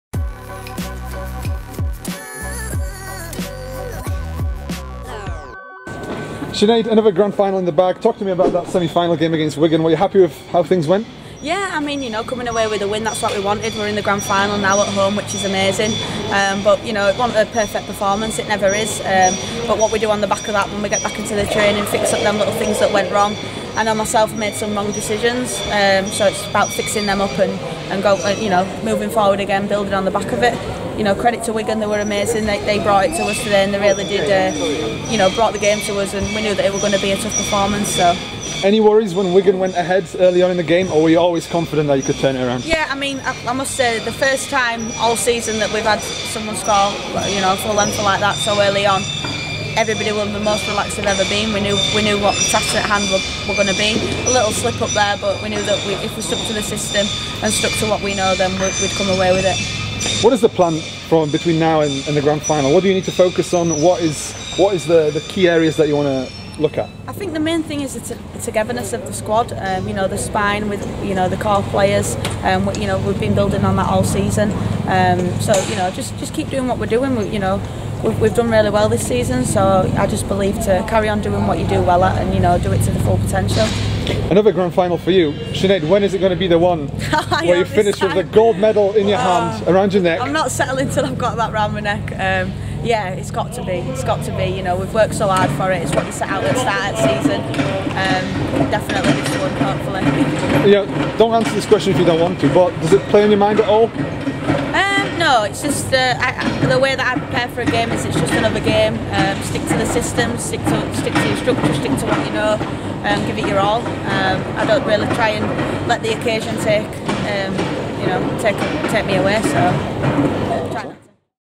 Sinead Peach REACTION to York Valkyrie booking Women's Super League GRAND FINAL spot!
Sinead Peach gives her immediate reaction after York Valkyrie beat Wigan Warriors 22-6 in the Women's Super League semi-final.

York will now take on Leeds Rhinos in the Grand Final - a repeat of the 2022 showpiece!